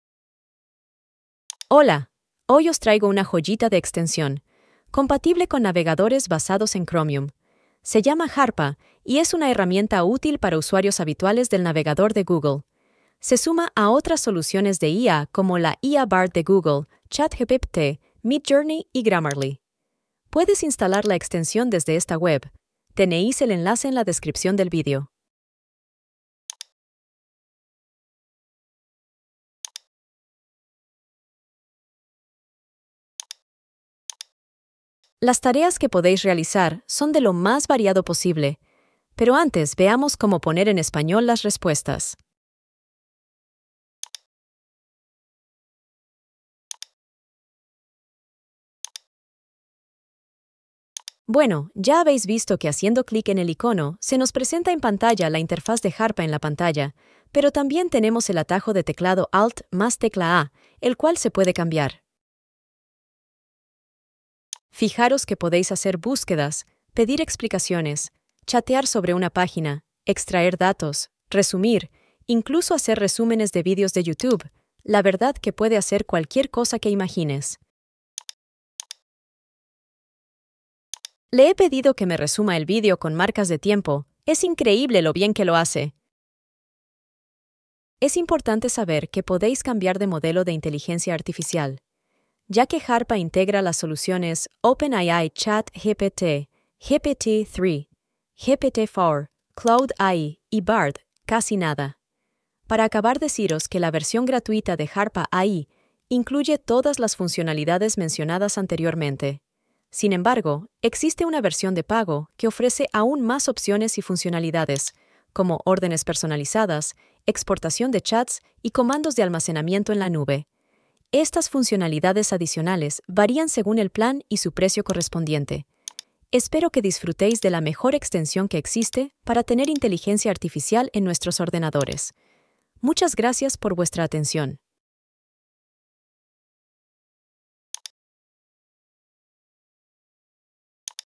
La mejor herramienta IA 🤖
🙋‍♂️Harpa: ¡No te pierdas esta herramienta que cambiará tu forma de navegar!

Harpa suena como una herramienta increíblemente útil para mejorar la productividad y la eficiencia mientras navegas por la web. Aquí hay un resumen de sus características principales:

¿Cómo instalar Harpa?

1. **Búsqueda de información precisa**: Harpa puede proporcionar respuestas precisas y relevantes a tus preguntas.

2. **Explicaciones claras**: Si encuentras algo que no entiendes, puedes pedirle a Harpa que te lo explique de manera más sencilla.

3. **Chat sobre una página web**: Puedes interactuar con Harpa mientras navegas para obtener información adicional o diferentes perspectivas sobre el contenido.

4. **Extracción de datos**: Harpa te permite extraer datos de páginas web y guardarlos en un formato útil para ti.

5. **Resúmenes de texto y videos**: Puedes solicitar resúmenes de texto o incluso de videos de YouTube, con marcas de tiempo para acceder rápidamente a la información relevante.

Para personalizar tu experiencia, puedes cambiar entre diferentes modelos de IA, como ChatGPT, GPT3/GPT4, ClaudeAI y Bard. Además, mientras que la versión gratuita ofrece muchas funciones útiles, la versión de pago ofrece opciones adicionales como órdenes personalizadas y almacenamiento en la nube.

Para instalar Harpa, puedes seguir el enlace proporcionado en la publicación que mencionaste. Recuerda experimentar con los diferentes modelos de IA, utilizar atajos de teclado para ahorrar tiempo y personalizar la configuración según tus necesidades.

En resumen, Harpa es una herramienta poderosa que puede mejorar tu experiencia de navegación y aumentar tu productividad en línea.